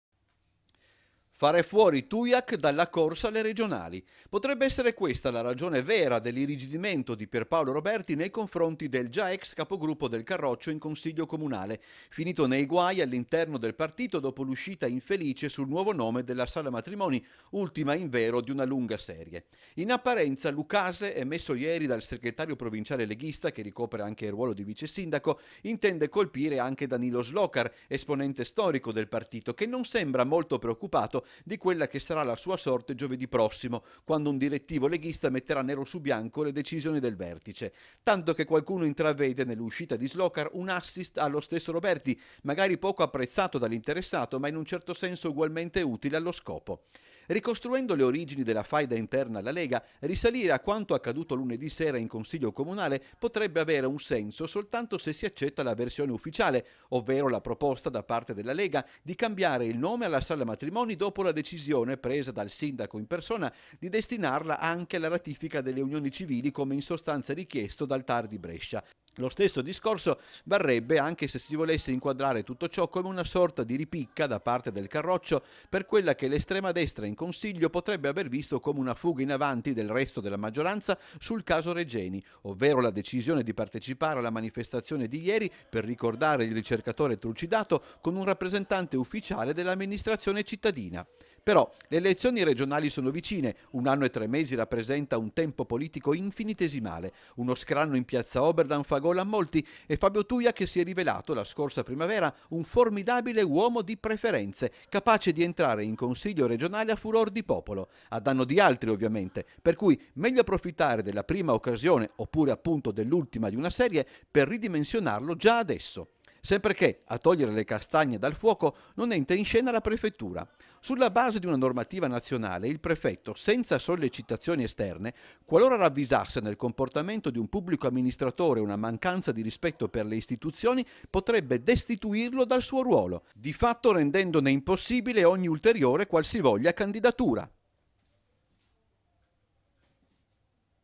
26/01/2017 - LEGA TUIACH: OBIETTIVO? NON CANDIDARLO ALLE REGIONALI
IL NOTIZIARIO (giovedì 26 gennaio 2017) - Ci sarebbe un piano alla base del diktat di Polidori contro Ttuiach, dopo l'uscita sul nuovo nome alla Sala Matrimoni.